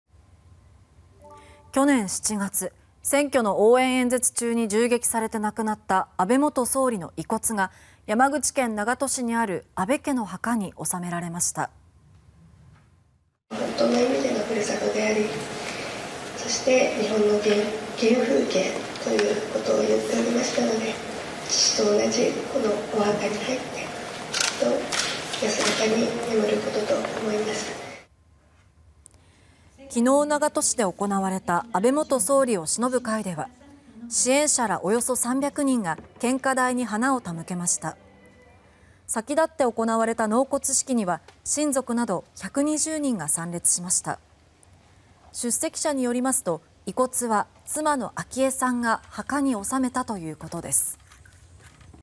山口・長門市の墓所へ 安倍元総理の納骨式(2023年7月24日)
去年7月、選挙の応援演説中に銃撃されて亡くなった安倍元総理の遺骨が山口県長門市にある安倍家の墓に納められました。

　安倍昭恵さん：「本当の意味での故郷。日本の原風景だと言っておりました。父と同じお墓に入って、きっと安らかに眠ることと思います」

　23日、長門市で行われた安倍元総理をしのぶ会では、支援者ら約300人が献花台に花を手向けました。

　先だって行われた納骨式には、親族など120人が参列しました。

　出席者によりますと、遺骨は妻の昭恵さんが墓に納めたということです。